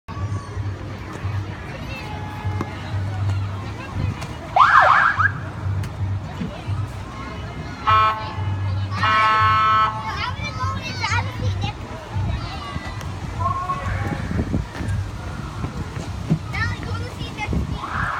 Katie David and Natalie are exploring the fire truck
They had a lot of fun that day. L